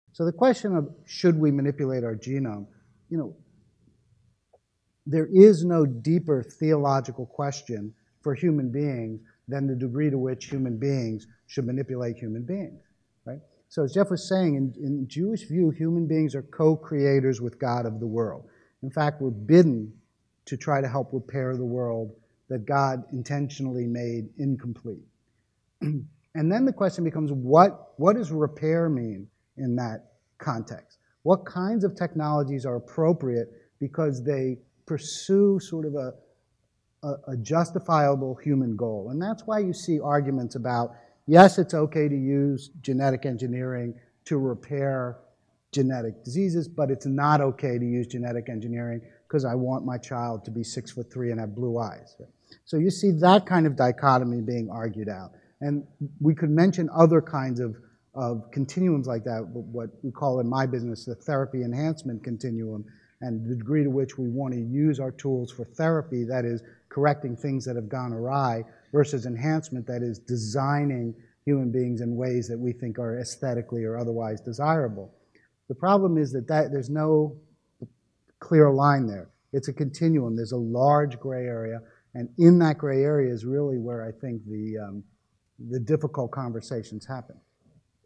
Should we manipulate our genome? Professor Ian Hutchinson asks the question
Discover more about our partner here:

GBH Forum Network ~ Free online lectures: Explore a world of ideas